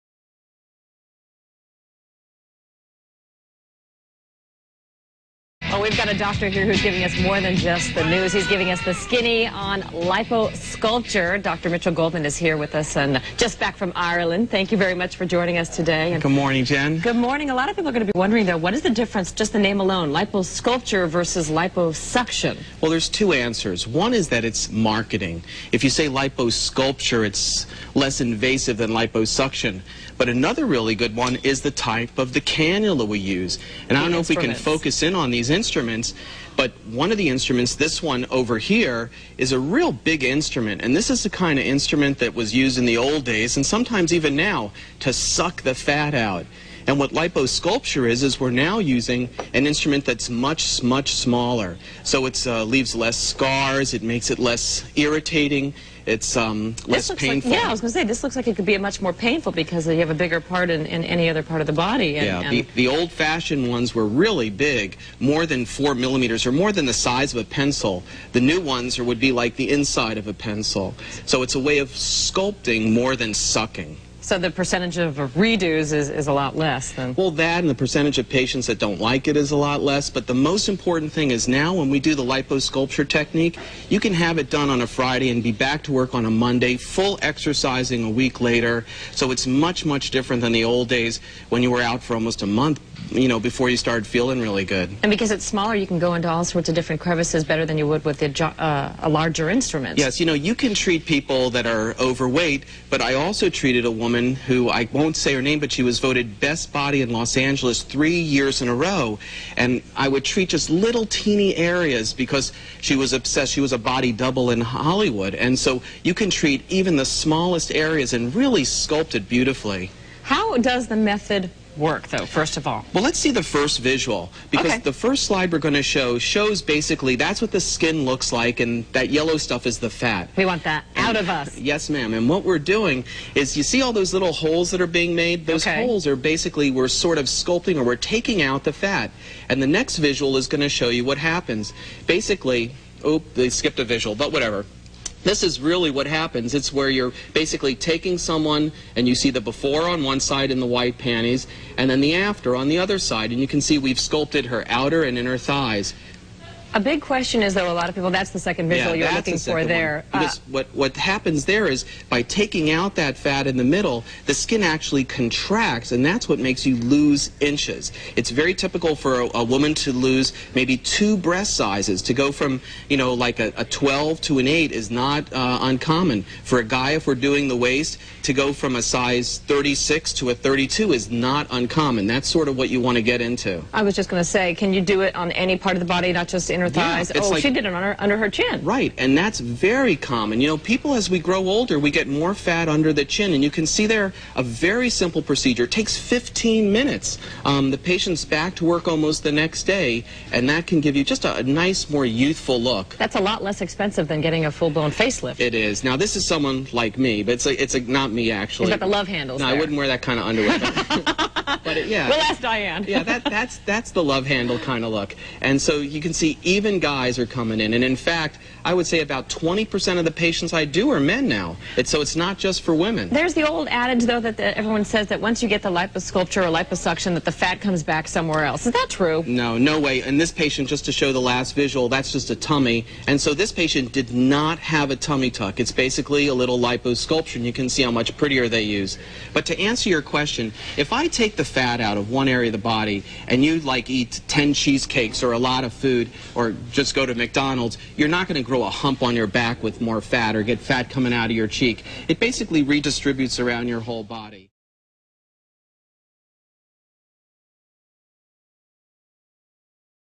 What is Liposculpture? | Liposculpture vs. Liposuction | Dr. Goldman
San Diego dermatologist Mitchel Goldman, MD talks about the benefits of liposculpture treatments for stubborn fat reduction. He explains the differences between liposculpture and liposuction.